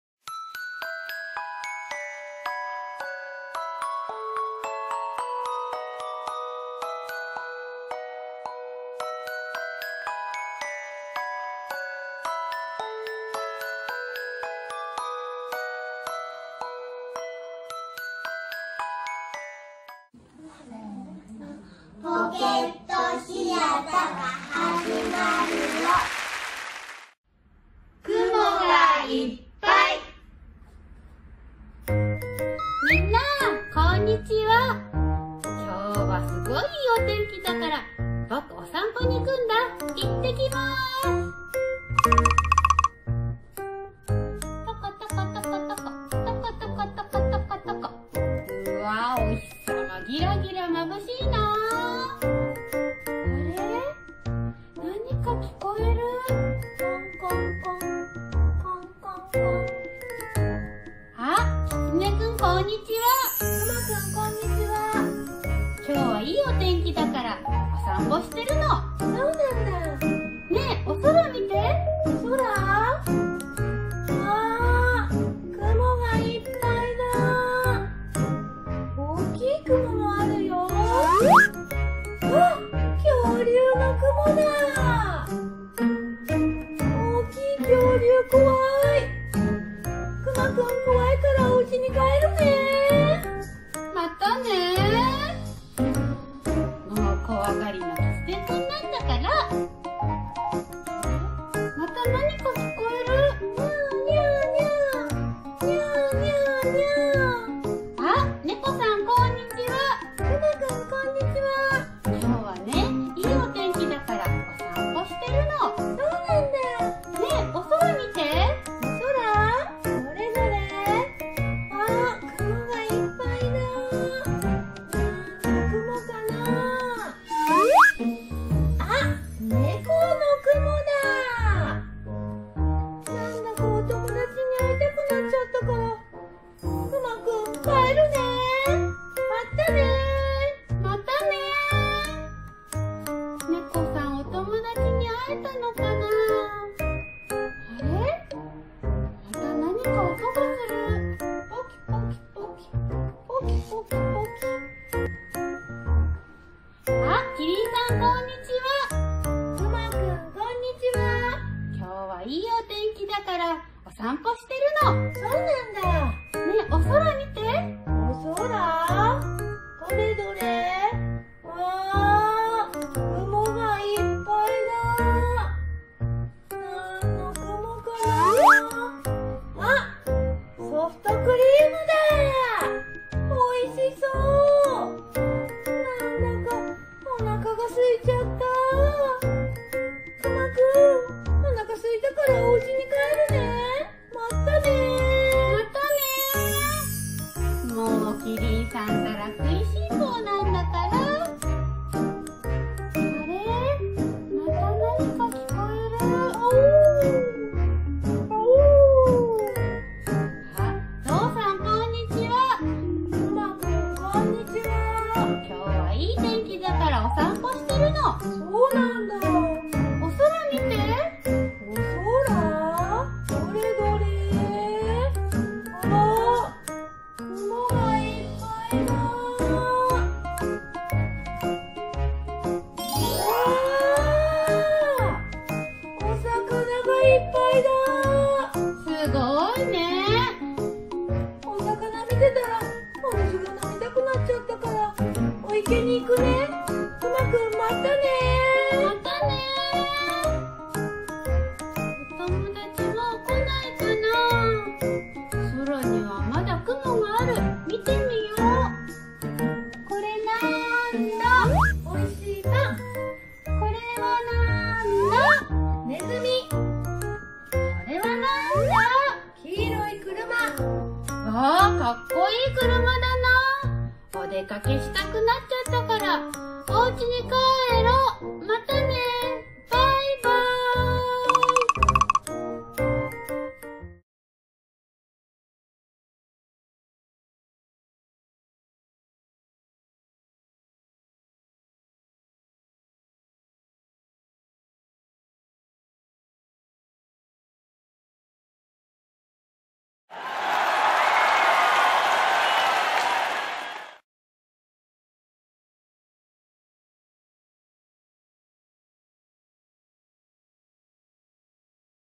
ぽけっと２１シアター「くもがいっぱい 」
市内７か所にある子育て支援センター「ぽけっと２１」は、親子で自由に使うことができるフリースペースです。
今回は、ぽけっと２１のアドバイザーが「くもがいっぱい」をテーマにしたベープサートを作成しました。
親子で一緒にご覧ください。